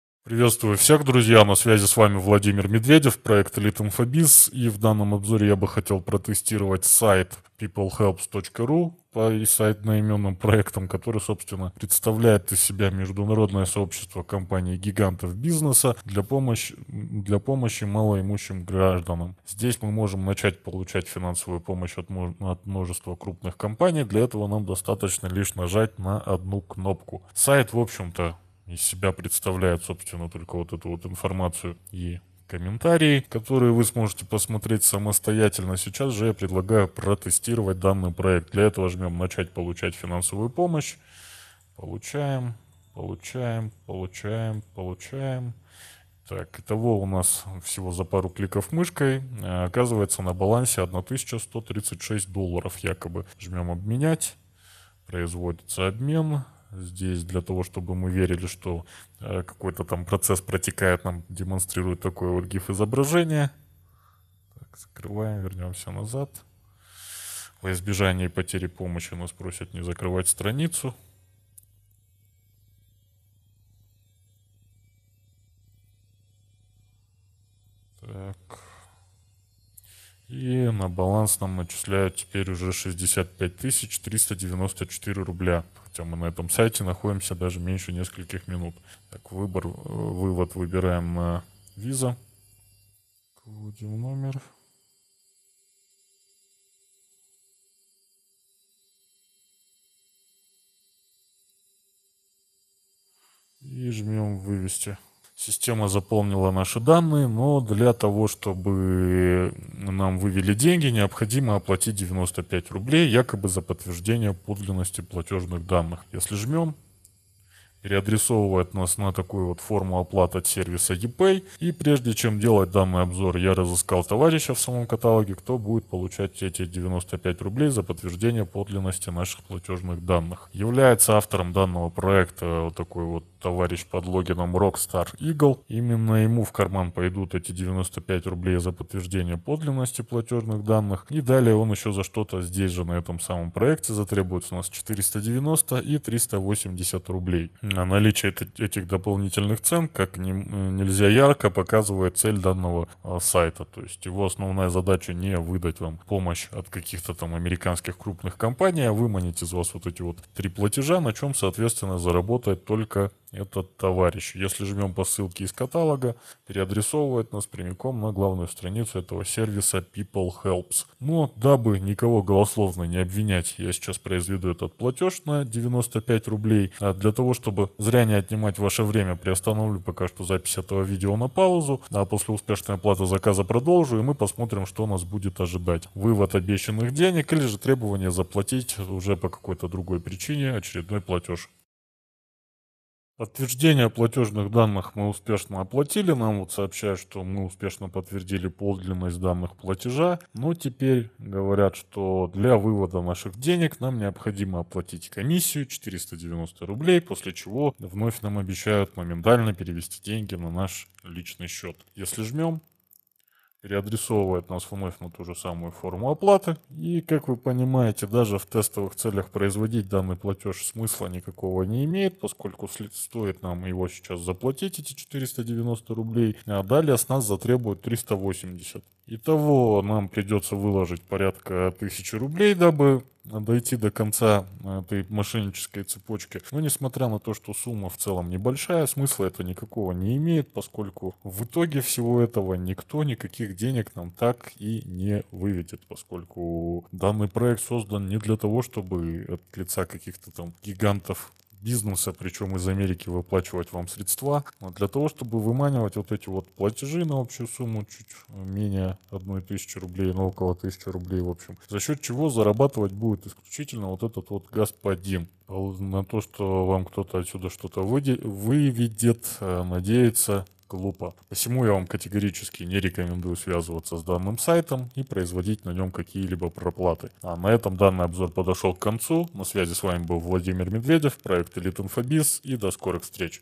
People-helps.ru окажет вам финансовую помощь от множества крупных компаний? Честный отзыв.
На мошенническом сайте от лица одноименного сервиса People-helps.ru всем желающим предлагают получать финансовую помощь от множества крупных компаний. И даже за пару кликов мышкой дадут вам посмотреть на ваш "заработок". Увы вывести вы его так и не сможете, поскольку задача проекта вводить людей в заблуждение и выманивать деньги, а не предоставлять заработок. Смотрите наш обзор и не попадайтесь на этот лохотрон!